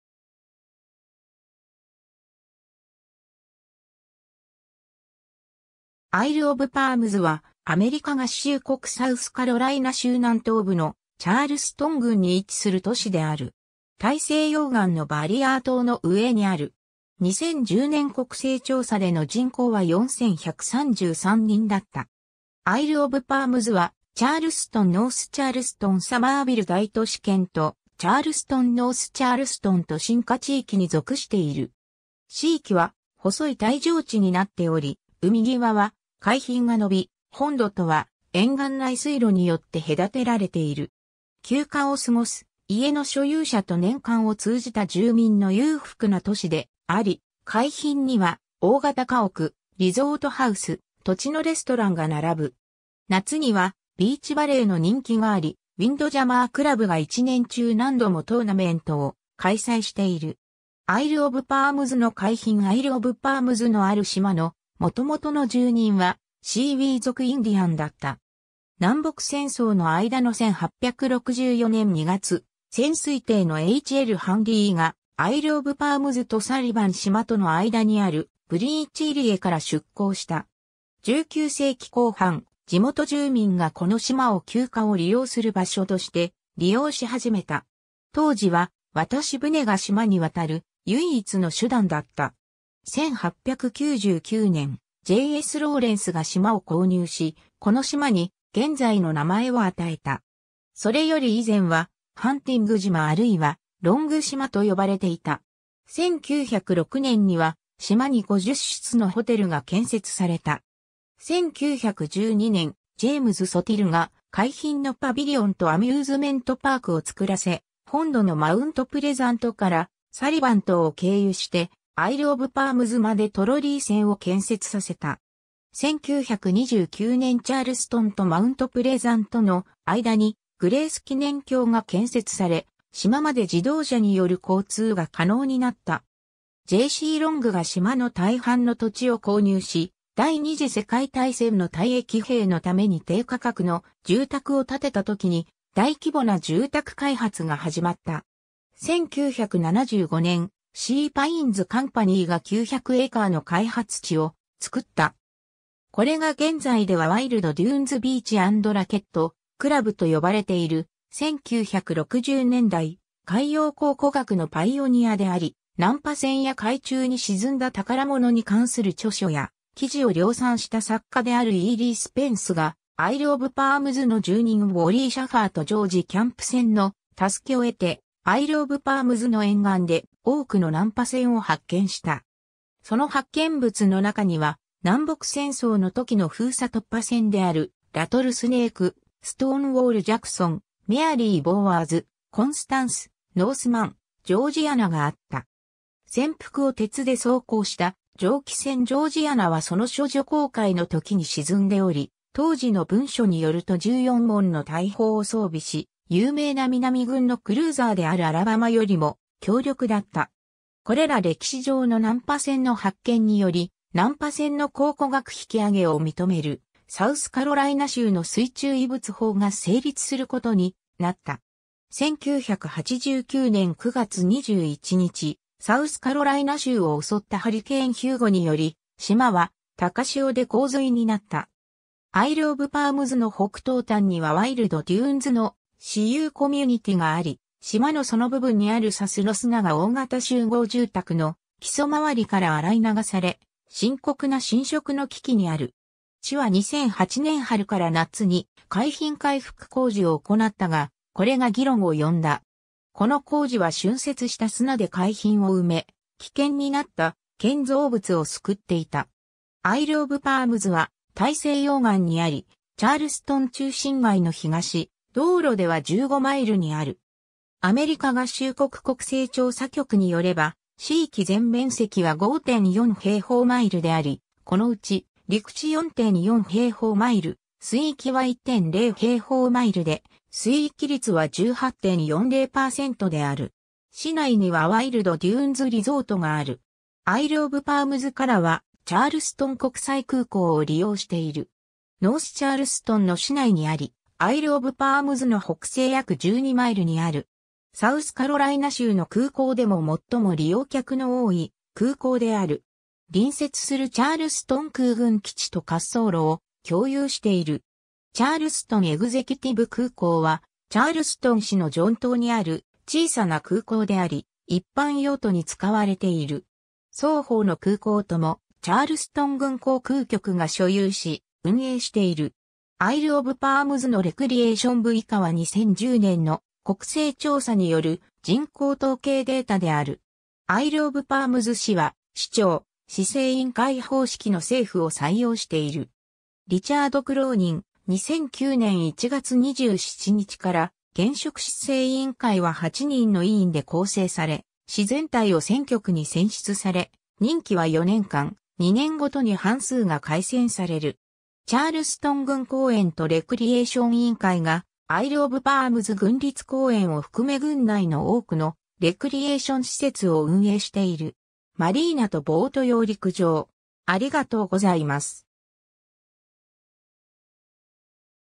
アイルオブパームズ (サウスカロライナ州)
チャールストン郡_(サウスカロライナ州)
アイルオブパームズ（英: Isle of Palms）は、アメリカ合衆国サウスカロライナ州南東部のチャールストン郡に位置する都市である。
大西洋岸のバリアー島の上にある。
2010年国勢調査での人口は4,133 人だった。
アイルオブパームズはチャールストン・ノースチャールストン・サマービル大都市圏と、チャールストン・ノースチャールストン都心化地域に属している。
市域は細い帯状地になっており、海際は海浜が伸び、本土とは沿岸内水路によって隔てられている。
休暇を過ごす家の所有者と年間を通じた住民の裕福な都市であり、海浜には大型家屋、リゾートハウス、土地のレストランが並ぶ。
夏にはビーチバレーの人気があり、「ウィンドジャマー」クラブが一年中何度もトーナメントを開催している。
 アイルオブパームズの海浜 アイルオブパームズのある島の元々の住人はシーウィー族インディアンだった。
 南北戦争の間の1864年2月、潜水艇のH・L・ハンリーが、アイルオブパームズとサリバン島との間にあるブリーチ入り江から出港した。
 19世紀後半、地元住民がこの島を休暇を利用する場所として利用し始めた。
当時は渡し船が島に渡る唯一の手段だった。
1899年、J・S・ローレンスが島を購入し、この島に現在の名前を与えた。
それより以前は「ハンティング島」あるいは「ロング島」と呼ばれていた。
1906年には、島に50室のホテルが建設された。
1912年、ジェイムズ・ソティルが海浜のパビリオンとアミューズメントパークを作らせ、本土のマウントプレザントからサリバン島を経由してアイルオブパームズまでトロリー線を建設させた。
1929年チャールストンとマウントプレザントの間にグレース記念橋が建設され、島まで自動車による交通が可能になった。
 J・C・ロングが島の大半の土地を購入し、第二次世界大戦の退役兵のために低価格の住宅を建てたときに、大規模な住宅開発が始まった。
1975年、シーパインズ・カンパニー（ヒルトンヘッドアイランドの開発で著名）が900エーカー (3.60 km2) の開発地を造った。
これが現在ではワイルド・デューンズ・ビーチ・アンド・ラケット・クラブと呼ばれている、 1960年代、海洋考古学のパイオニアであり、難破船や海中に沈んだ宝物に関する著書や記事を量産した作家であるE・リー・スペンスが、アイルオブパームズの住人ウォリー・シャファーとジョージ・キャンプセンの助けを得て、アイルオブパームズの沿岸で多くの難破船を発見した。
その発見物の中には、南北戦争のときの封鎖突破船であるラトルスネーク、ストーンウォール・ジャクソン、メアリー・ボウワーズ、コンスタンス、ノースマン、ジョージアナがあった。
船腹を鉄で装甲した蒸気船ジョージアナはその処女航海の時に沈んでおり、当時の文書に拠ると14門の大砲を装備し、有名な南軍のクルーザーであるアラバマよりも強力だった。
これら歴史上の難破船の発見により、難破船の考古学引き上げを認めるサウスカロライナ州の水中遺物法が成立することになった。
 1989年9月21日、サウスカロライナ州を襲ったハリケーン・ヒューゴにより、島は高潮で洪水になった。
 アイルオブパームズの北東端にはワイルド・デューンズの私有コミュニティがあり、島のその部分にある砂州の砂が大型集合住宅の基礎周りから洗い流され、深刻な浸食の危機にある。
市は2008年春から夏に海浜快復工事を行ったが、これが議論を呼んだ。
この工事は浚渫した砂で海浜を埋め、危険になった建造物を救っていた。
 アイルオブパームズは大西洋岸にあり、チャールストン中心街の東、道路では15マイル (24 km) にある。
アメリカ合衆国国勢調査局に拠れば、市域全面積は5.4平方マイル (14.1 km2)であり、このうち陸地4.4平方マイル (11.5 km2)、水域は1.0平方マイル (2.6 km2)で水域率は18.40%である。
 市内にはワイルド・デューンズ・リゾートがある。
 アイルオブパームズからはチャールストン国際空港（空港コードCHS）を利用している。
ノースチャールストンの市内にあり、アイルオブパームズの北西約12マイル (20 km) にある。
サウスカロライナ州の空港でも最も利用客の多い空港である。
隣接するチャールストン空軍基地と滑走路を共有している。
チャールストン・エグゼキュティブ空港はチャールストン市のジョ...